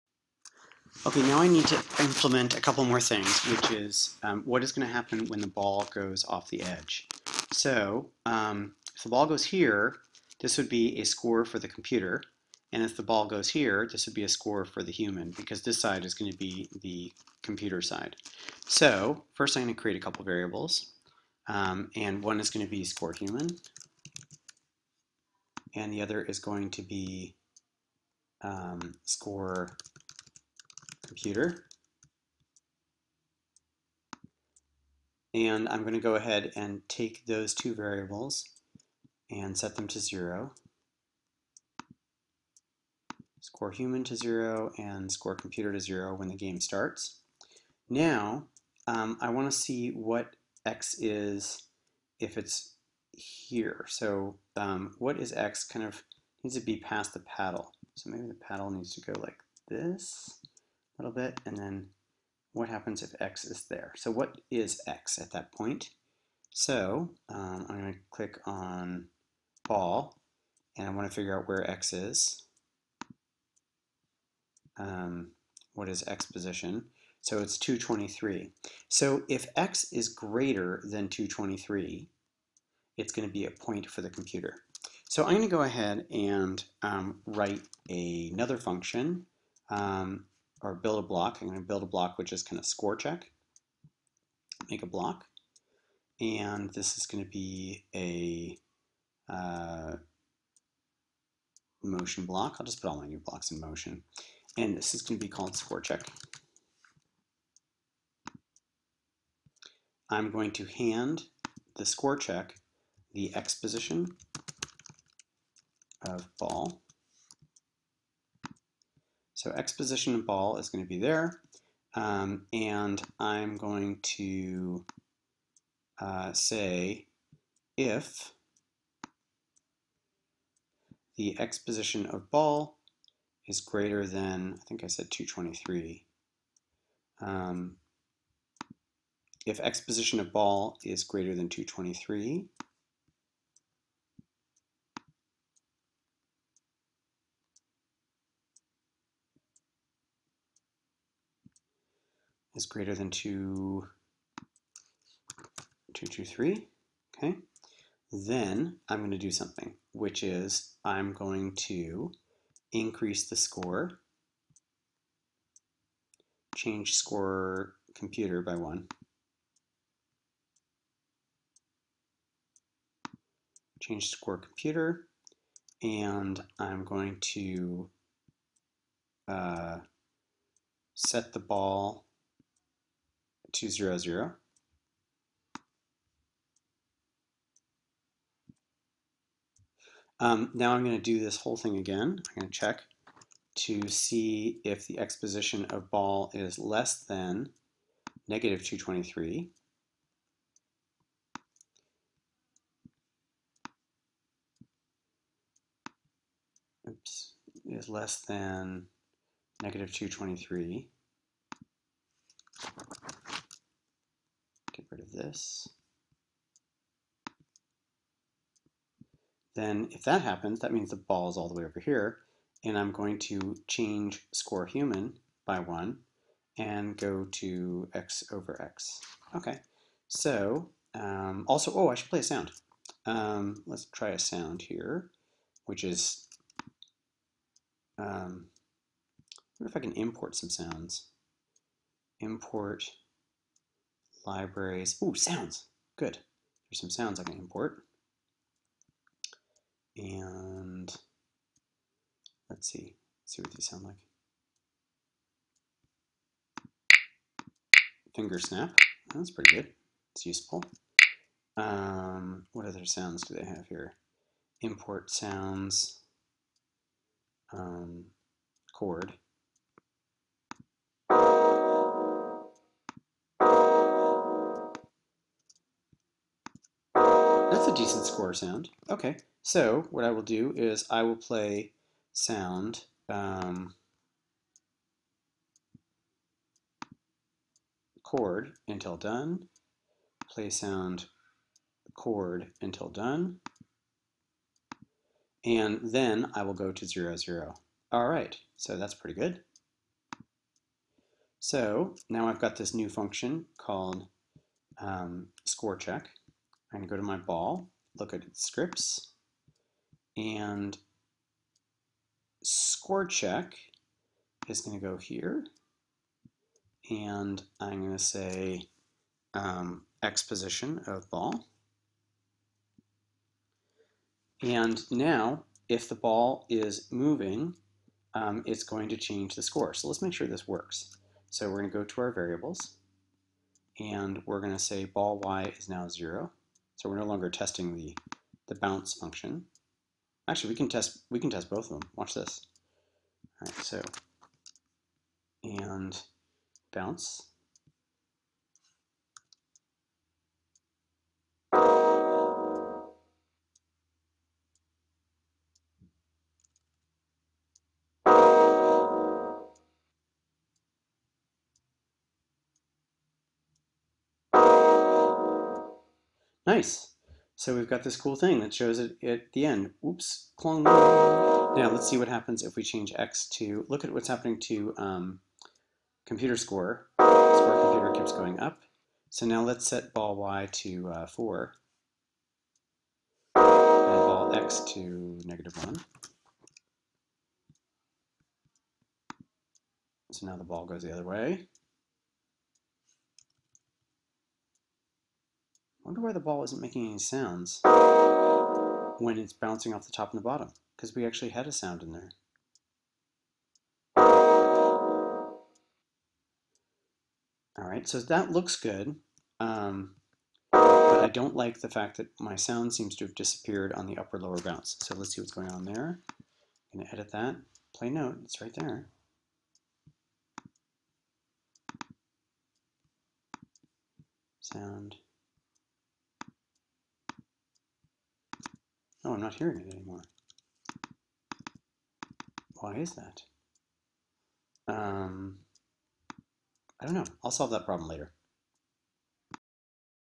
Implement score check if ball gets past paddle
Change the score if it gets past each paddle.